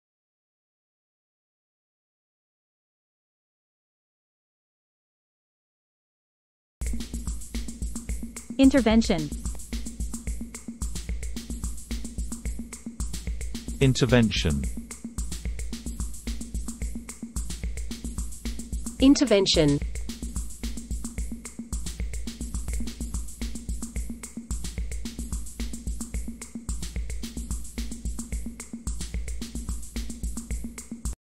Intervention ~ Pronounce Intervention in American Accent, Australian Accent, British Accent
"Intervention - Pronounce in Australian Accent, British Accent, American Accent along with it's meaning.

Meaning of the word Intervention is Interference(A noun)

How to pronounce Intervention in Australian Accent?

How to pronounce Intervention in British Accent?

How to pronounce Intervention in American Accent?"